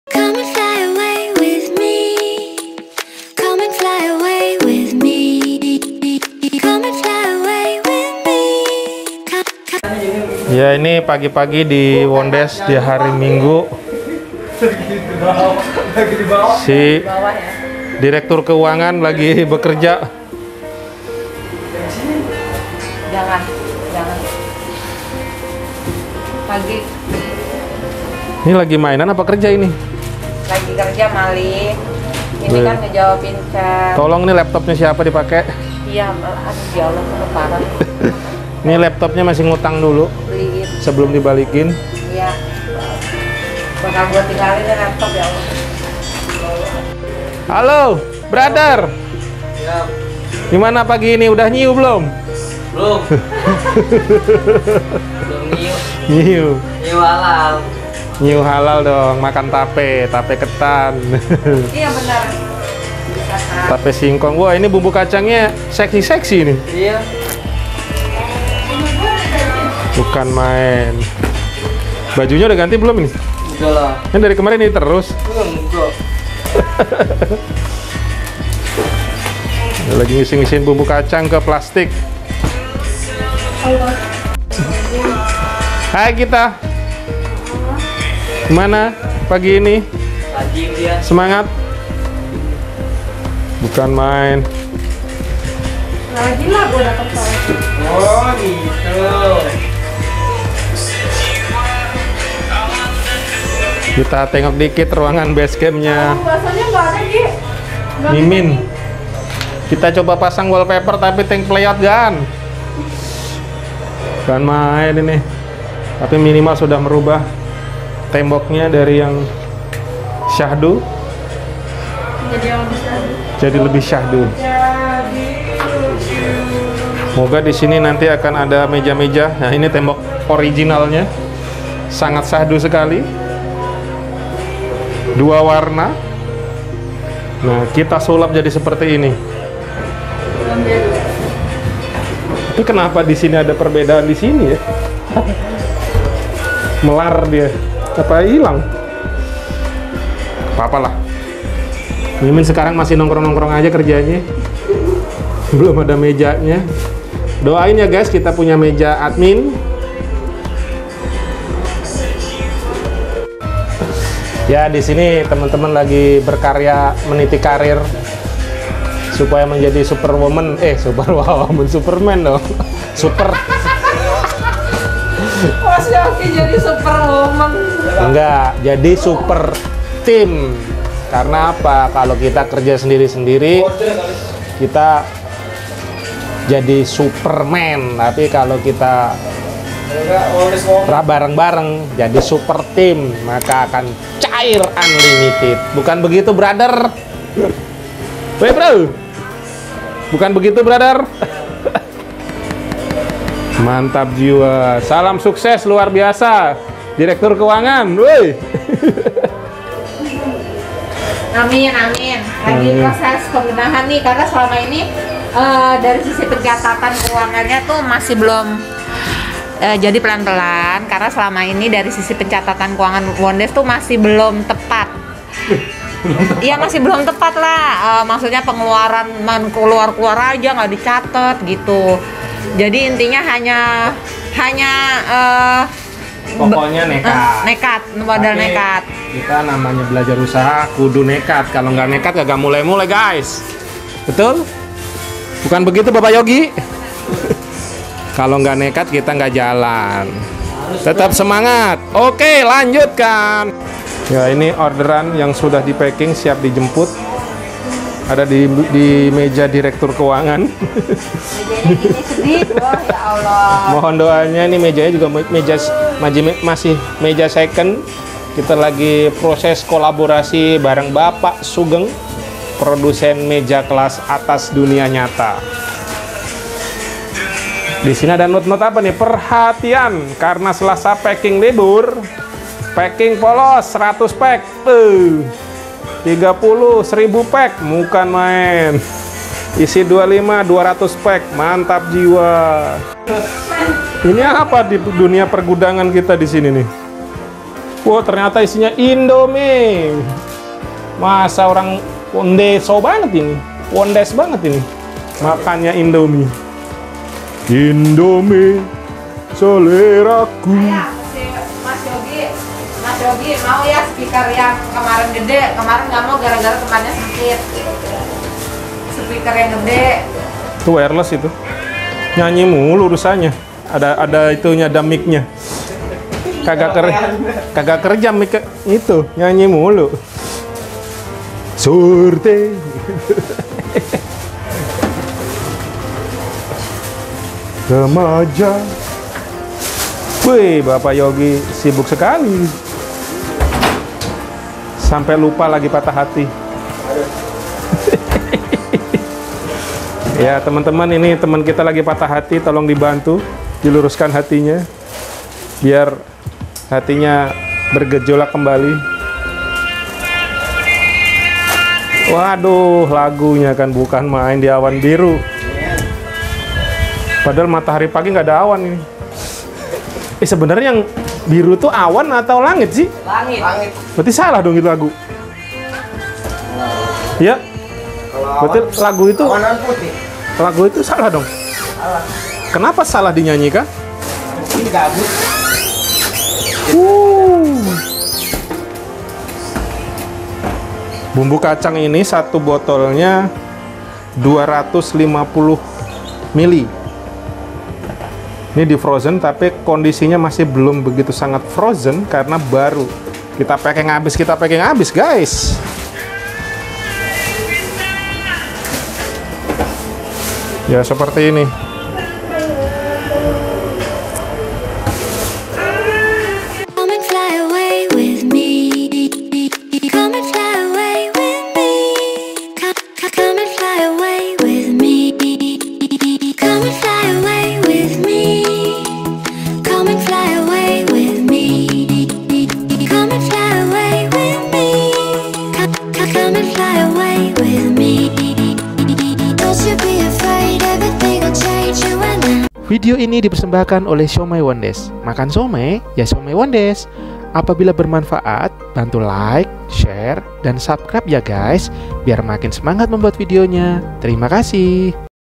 SUPERTEAM WONDES SEDANG BERKARYA UNLIMITED DI BASECAMP YANG BARU
Beginilah keseruan team wondes berkarya bareng menjemput rezeki